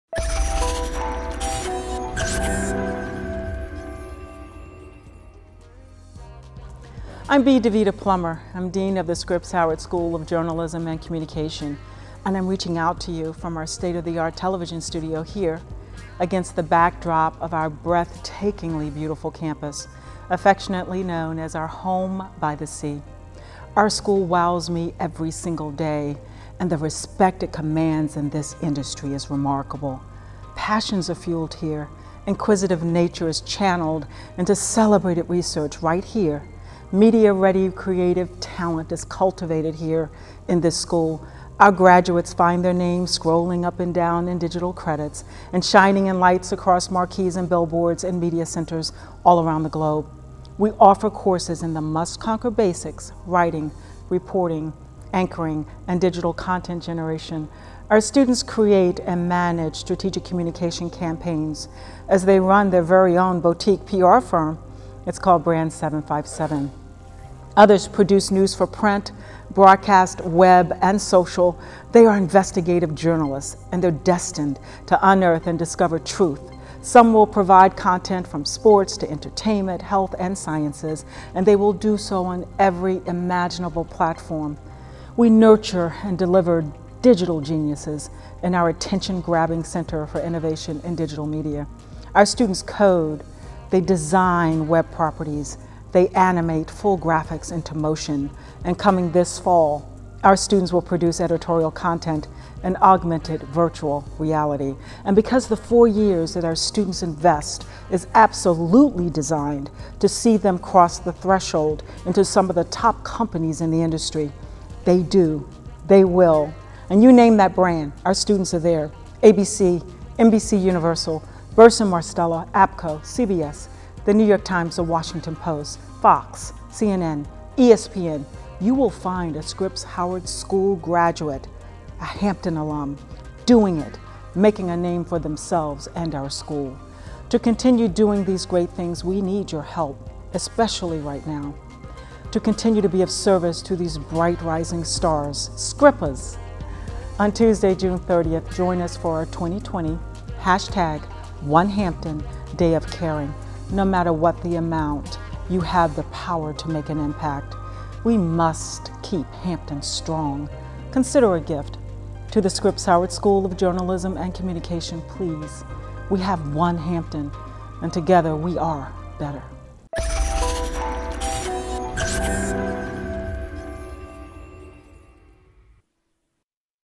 7 Reasons to Keep Hampton Strong: Academics at the School of Journalism and Communications
B. Davida Plummer, Dean of the Hampton University School of Journalism Communications, discusses how current programs offered prepare students for successful careers in news and media, and why supporting the School during the 2020 annual Day of Caring can have a major impact in developing the next generation of media leaders.

Join us June 30, 2020 and support our ‘Home by the Sea.’